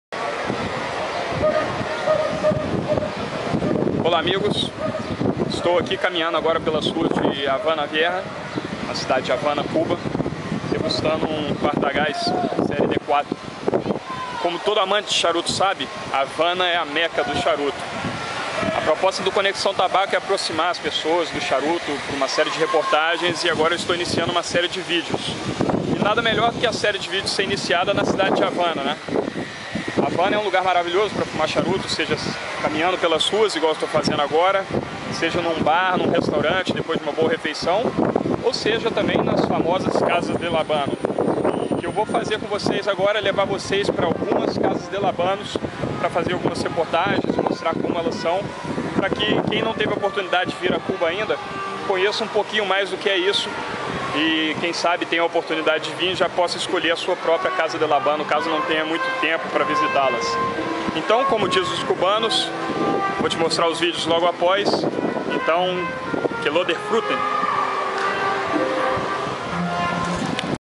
Conexão Tabaco - Reportagens LCDH - Indrodução
Vídeo de introdução à série de reportagens sobre as Casas del Habano de Cuba.